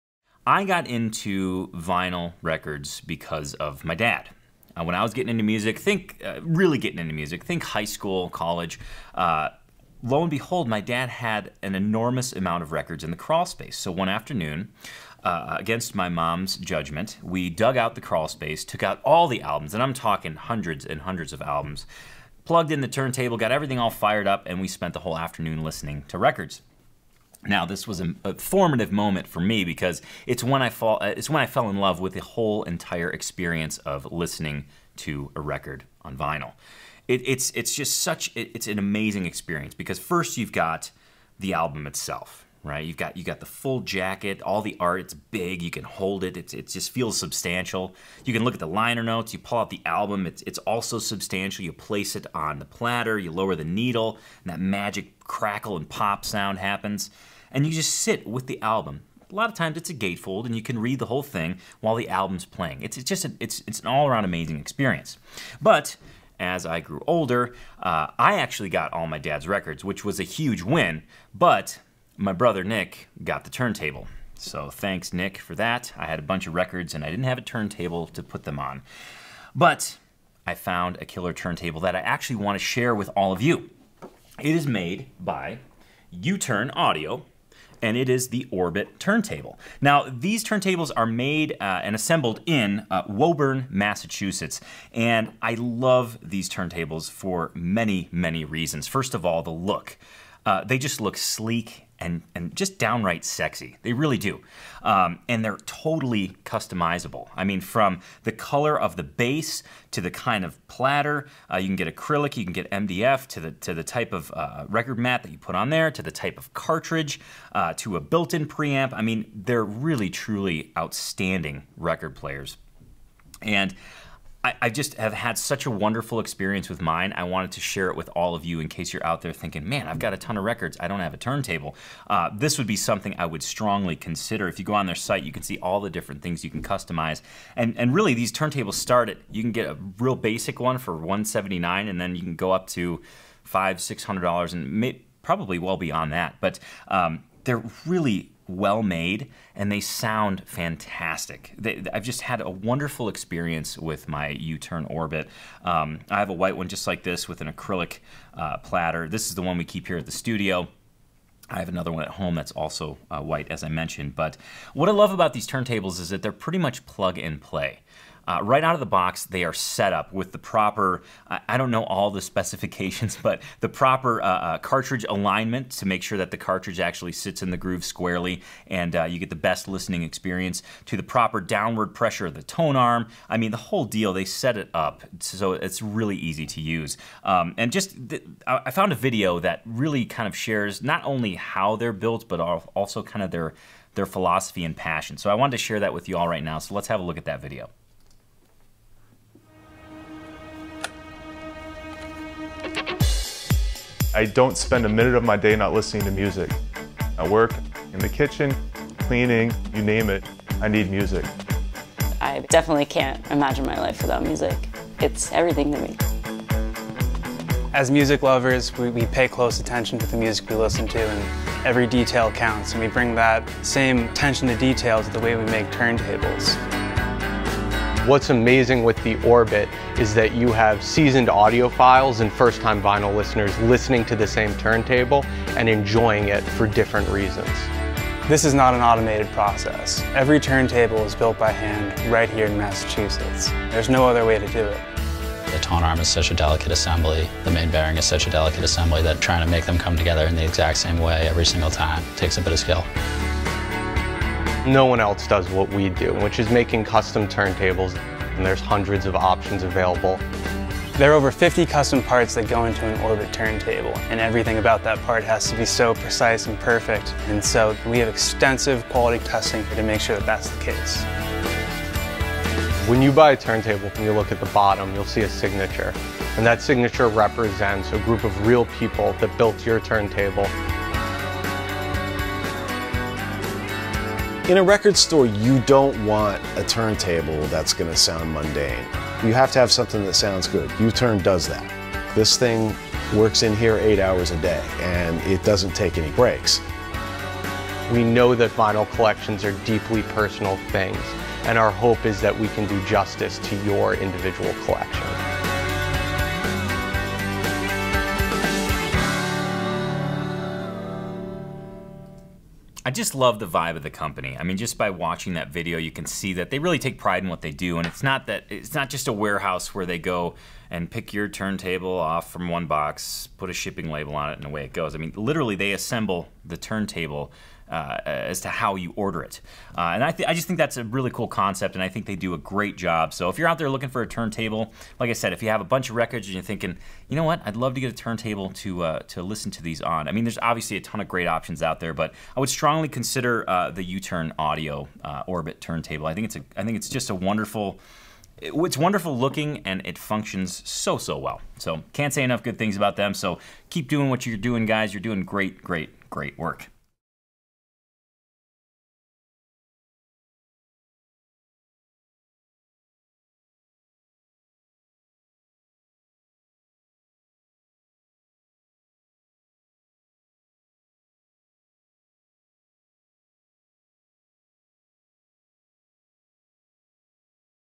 U-Turn Turntables Review
Vinyl albums are an item of pure beauty. From the album art, the shiny black disc, the liner notes, and the listening experience; vinyl records capture as much or more mojo and magic than the music was originally recorded with. The album is only half of the equation though... you need a turntable.

U-Turn Audio out of Woburn, MA is producing some remarkably beautiful turntables that function as amazing as they look.

The Orbit turntable made by the fine folks at U-Turn Audio is a fully customizable turntable that you can design to your specific music loving wants and needs. You can pick the color, platter style, cartridge type, and many more custom options making these turntables built exactly for you.

The pricing of these turntables starts at $179 and varies depending on the options you choose to add to the base model. In my experience, these turntables are some of the best bang for your buck record players out there.

A huge advantage of the U-Turn turntables is that they are ready to go right out of the box. The U-Turn team painstakingly sets up each turntable for maximum performance and consistency. They set the tone arm pressure, needle orientation, and every other tweak-able aspect of the record player so you can literally take it out of the box, plug it in, and have an incredible listening experience.

This turntable is a great first turntable for a newly converted vinyl connoisseur or even the experienced audiophile with a sensitive ear. Rest assured each turntable that comes out of the U-Turn shop is hand assembled and tested by a real human being... these folks are music lovers and they want you to have the best analog audio experience that you can.

Currently, there is an Orbit turntable in operation at Acoustic Life Studios as well as at my house... these are a treat to use and even more fun to listen to. If you have a pile of records that haven't
 been played in a while I would certainly do your research on U-turn, they may very well have the right turntable to bring your records back to life.